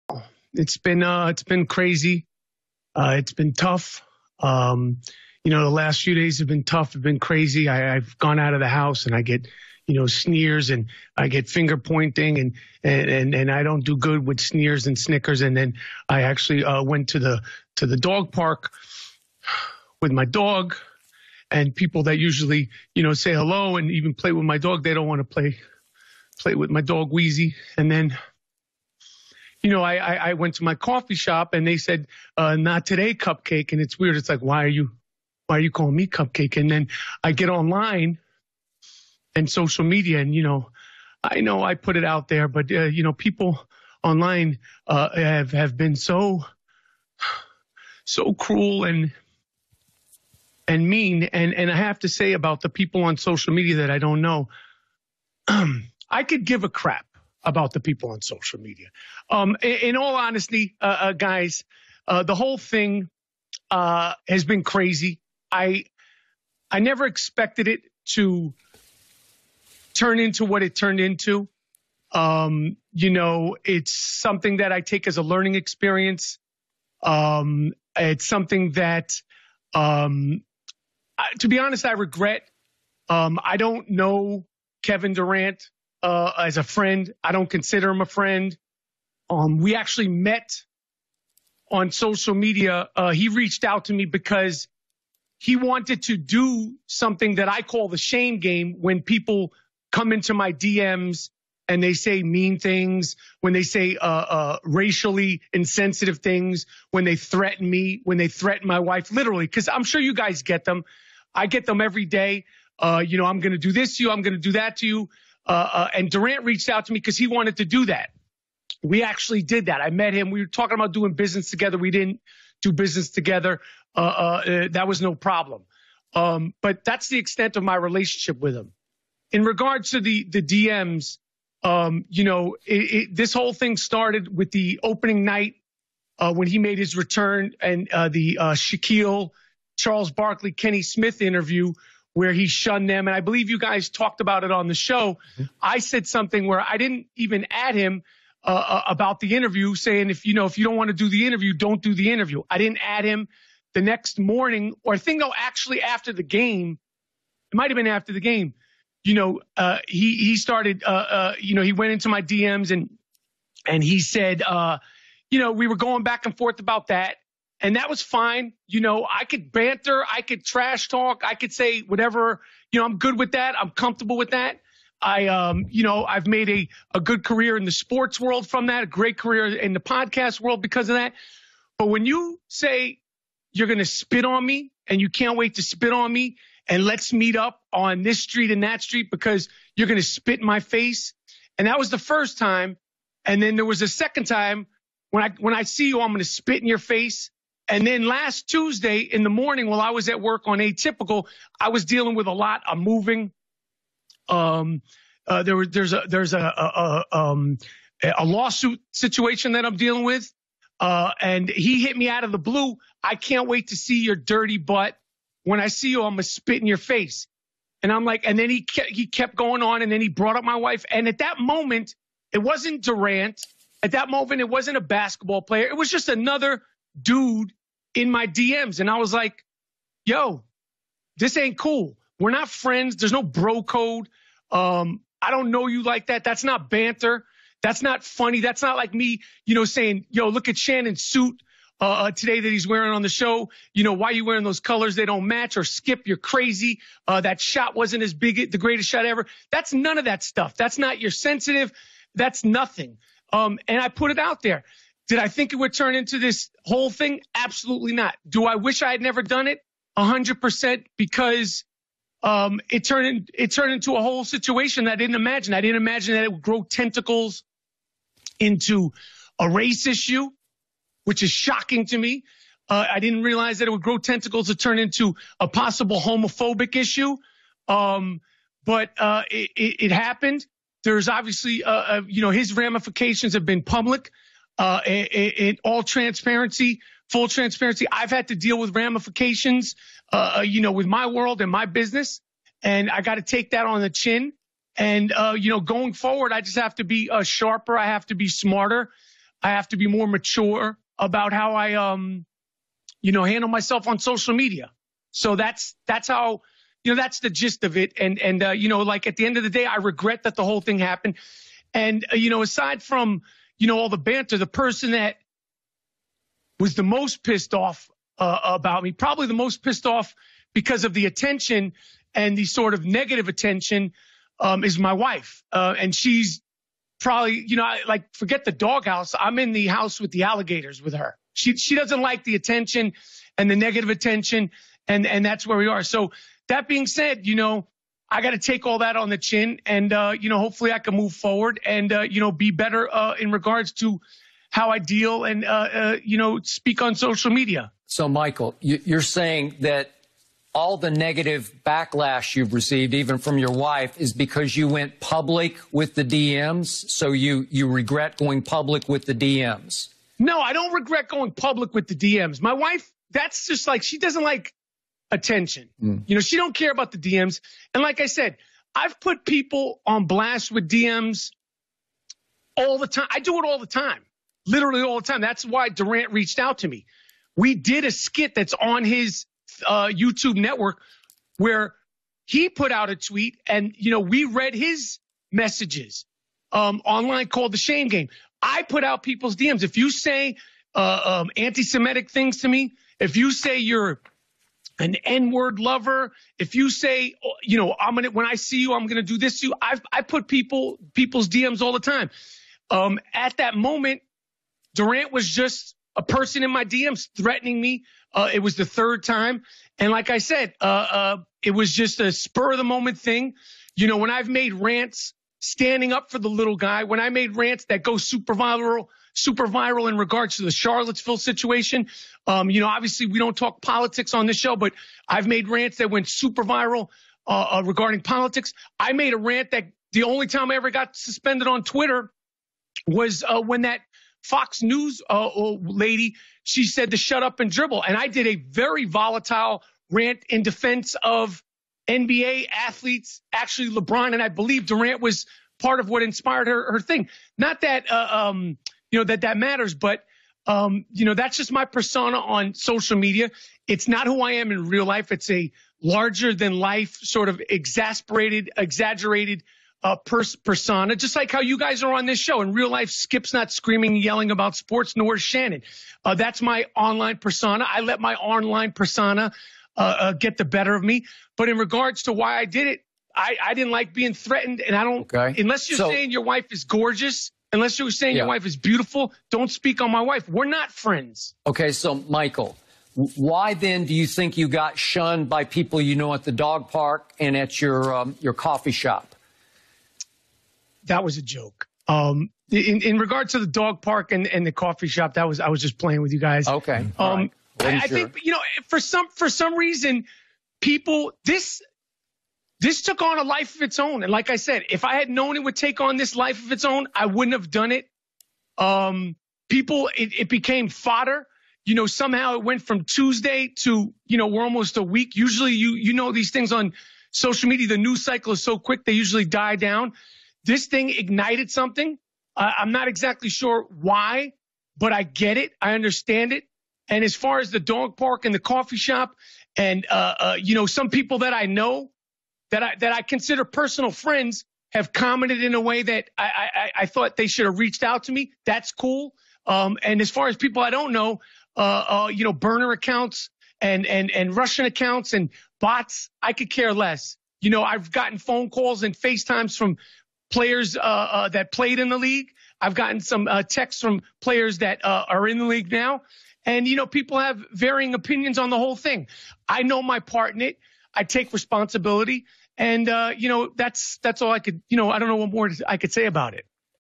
BREAKING NEANDERTHAL NEWS!!! 😭😭😭
Suspected Neanderthal Supremacist Michael Rapaport Snitches On Kevin Durant And Then Cries And Plays The Victim When He Is EXPOSED 😭😭😭